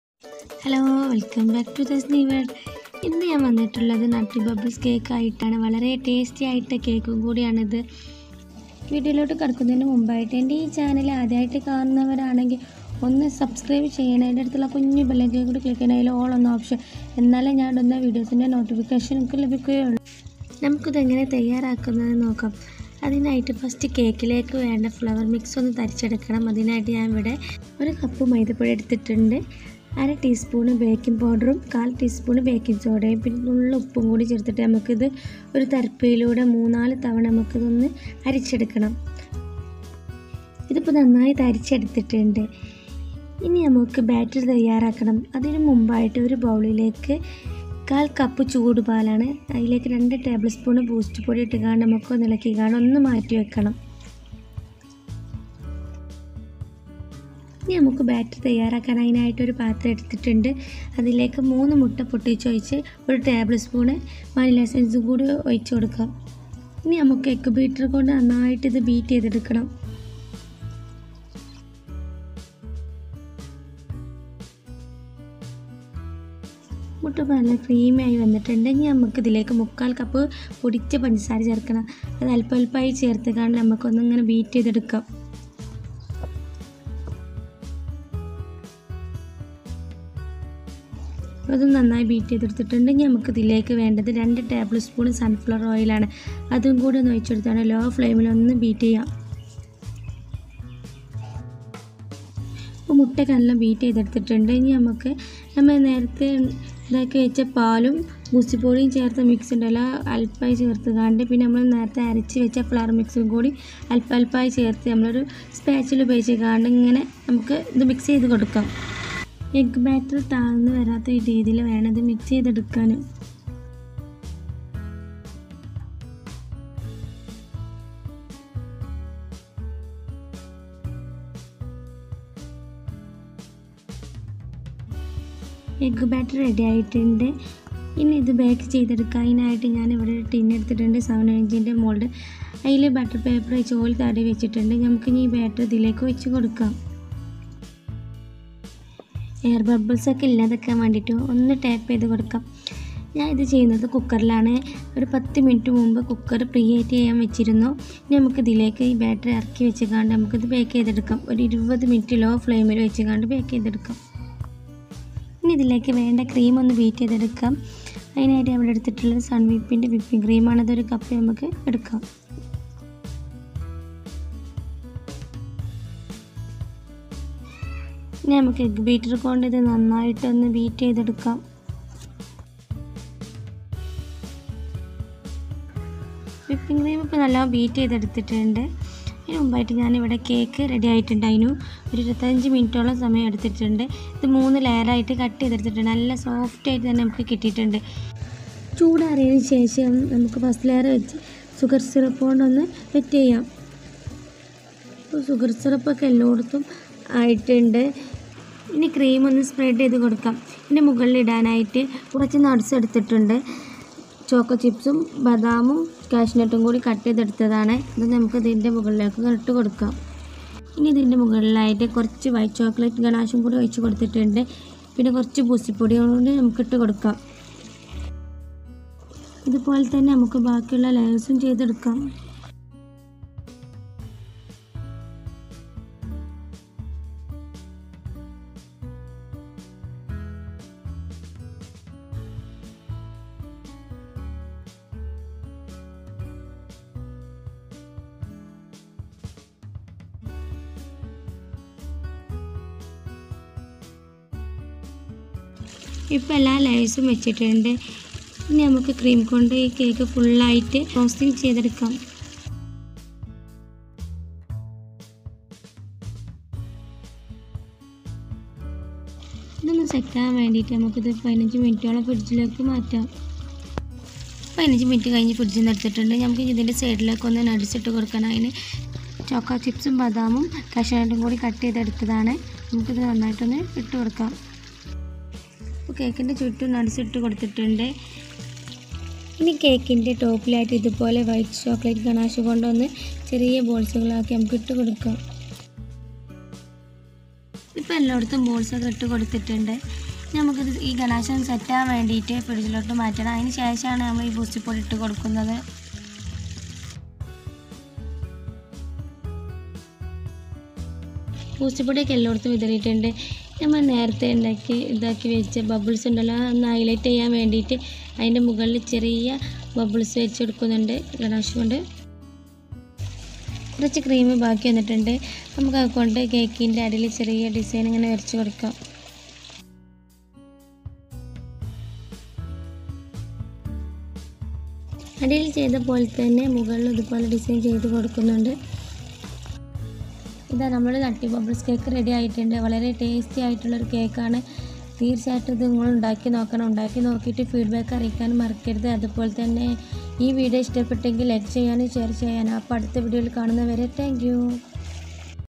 ഓവൻ ഇല്ലാതെ ഒരു perfect Nutty Bubble cake/ Home made/in malayalam
Ingredients
Egg -3
Flour -1 cup
Boost - tbspn

Badam
cashews
choco chips